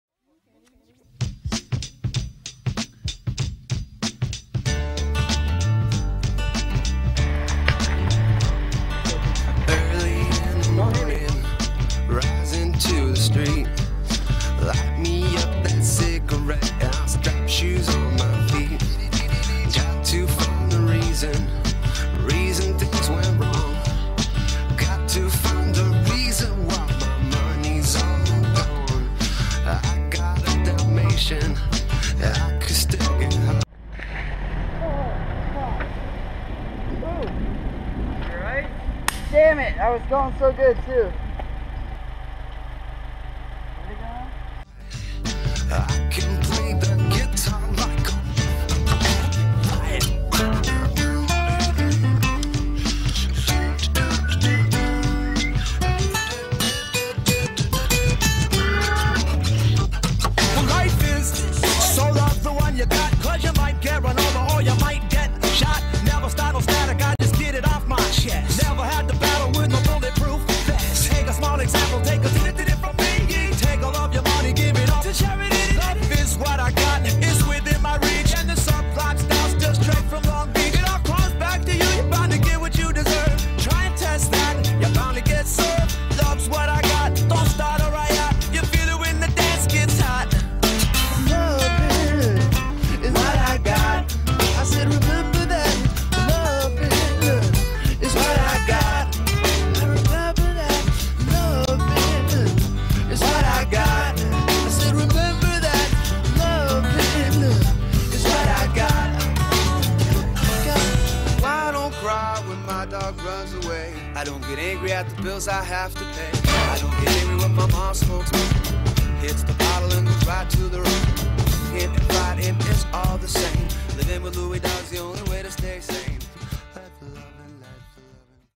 Longoard Skater Downhill Rider Liam Hayes and friends!
Hitting AT&T Hill with Liam Hayes, Colton Harrington & Jeffrey Vyain.